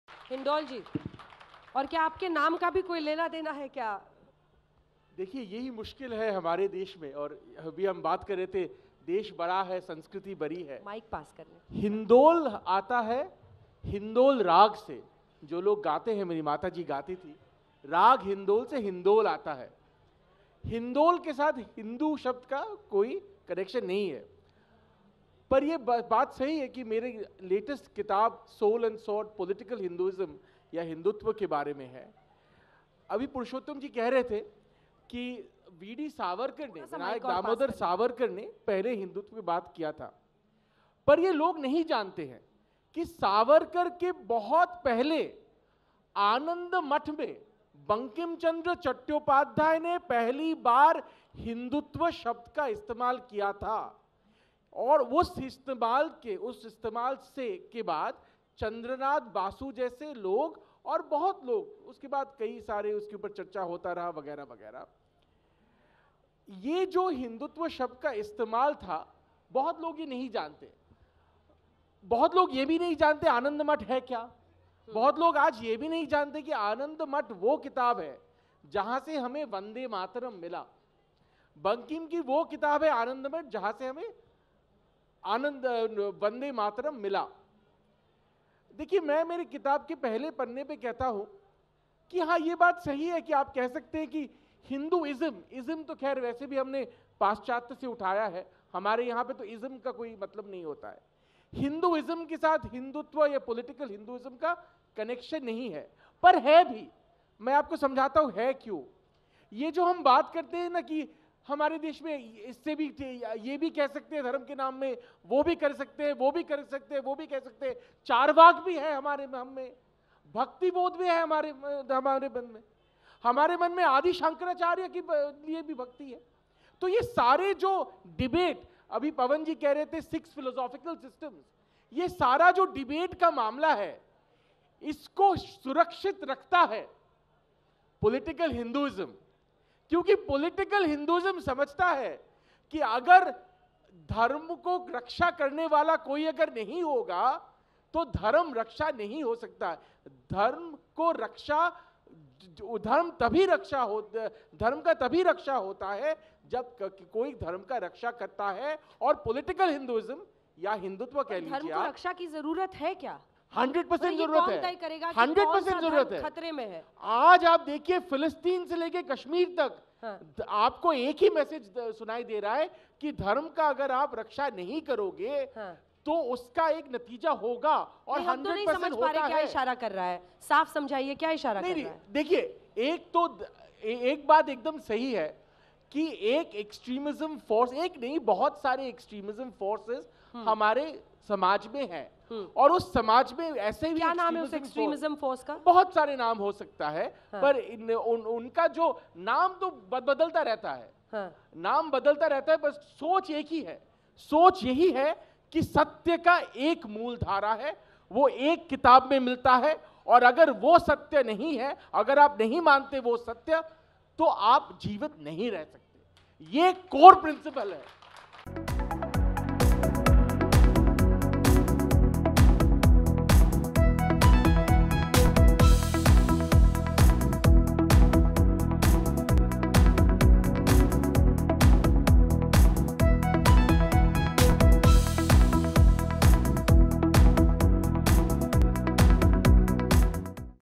Political Hinduism के बहाने Hindol Sengupta ने बता दिया- धर्म की रक्षा कौन करेगा? Sahitya Tak
Political Hinduism के बहाने लेखक हिंदोल सेनगुप्ता ने बता दिया- धर्म की रक्षा कौन करेगा? साहित्य तक पर सुनें यह चर्चा.